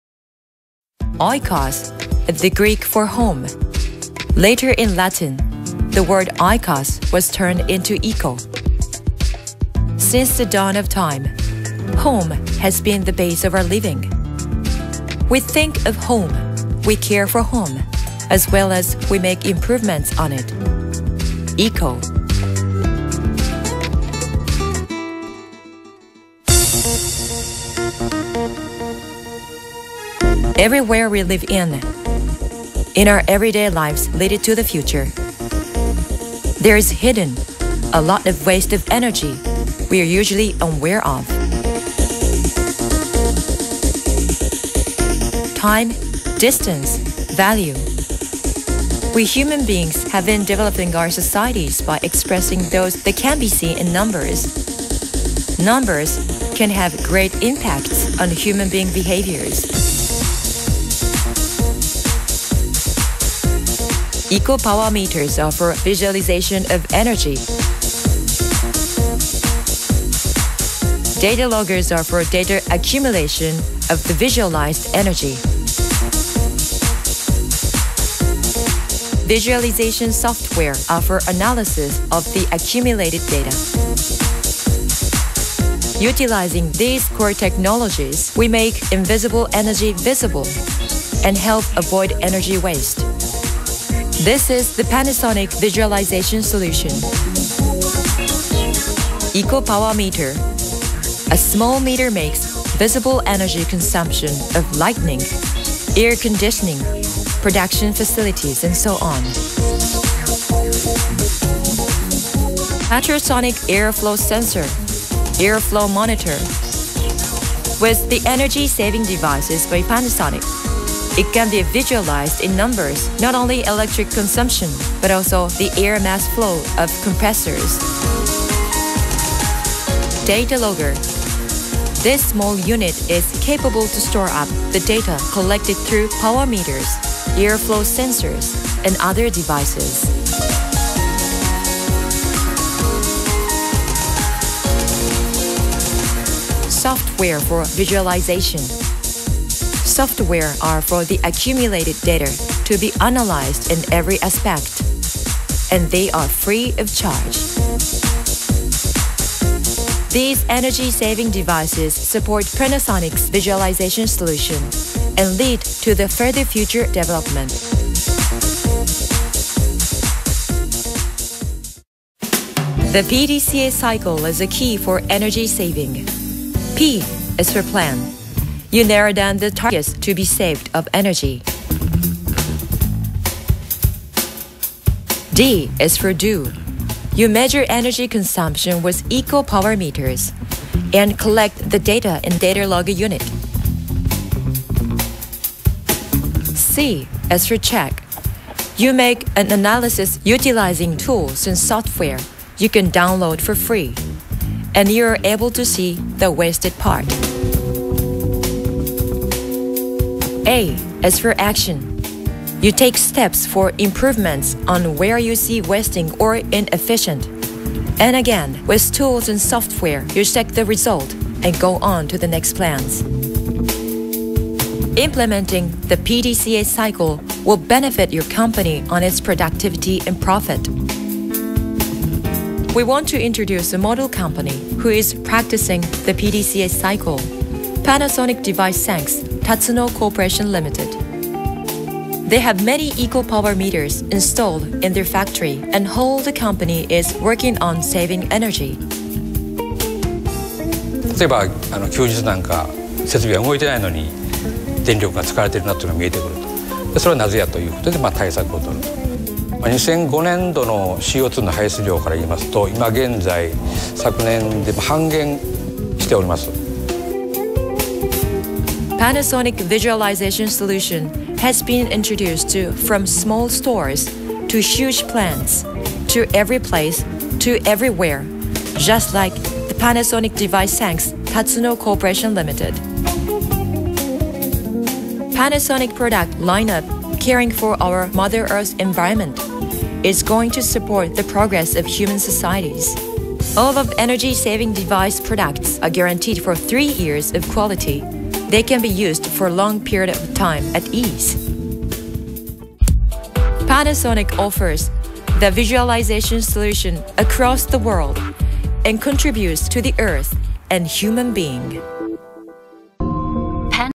ECO Visualization Solution
We introduce the need for Visualization and PDCA cycle for energy saving with Eco-POWER METER.